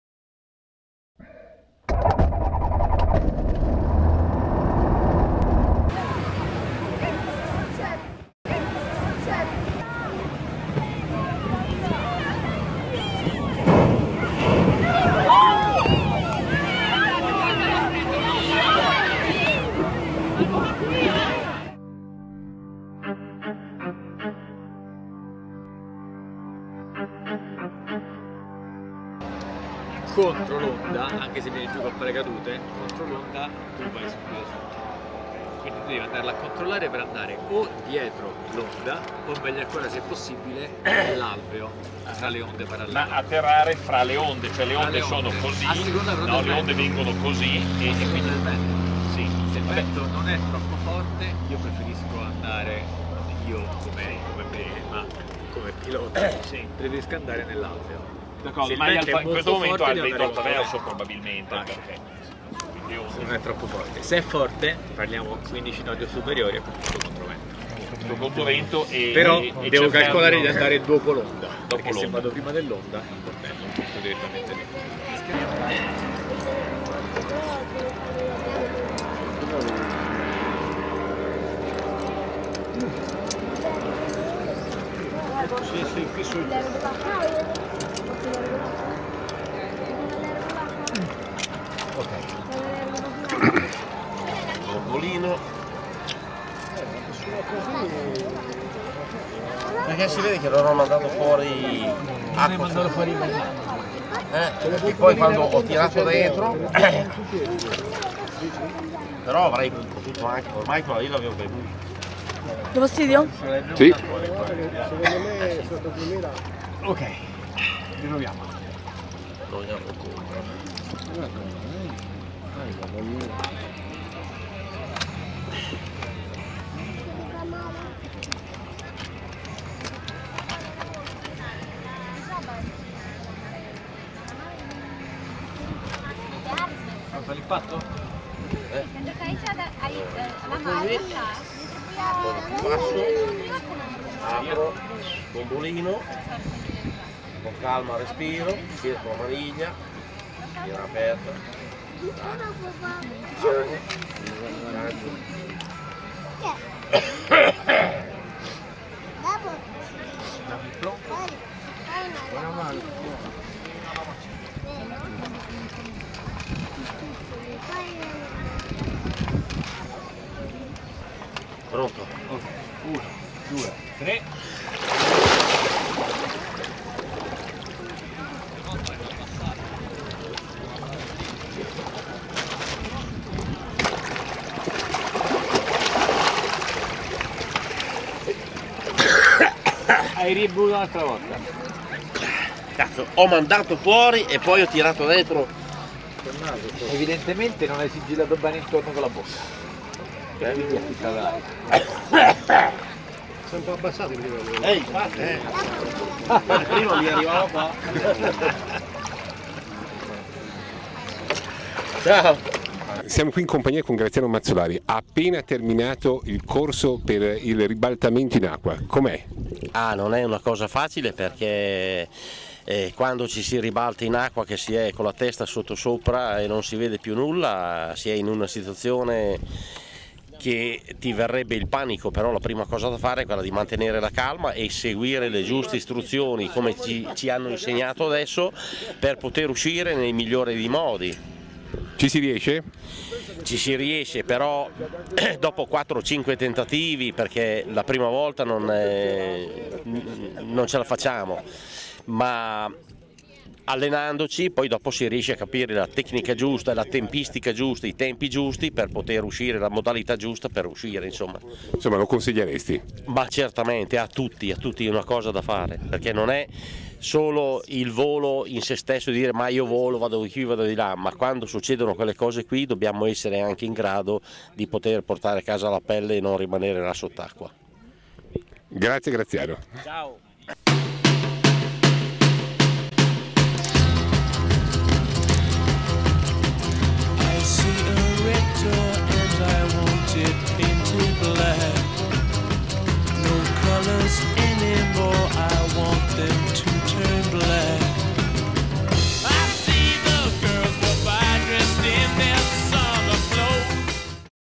AOPA VFRMeeting 2016 - Pavullo water egress
Dimostrazione delle tecniche per l'ammaraggio forzato, evacuazione da cabina sommersa e sopravvivenza in mare, svolto a Paulio in occasione del AOPA VFRMeeting 2016.
Intervisa a Graziano Mazzolari.
Servizio di: Enrico Carpineti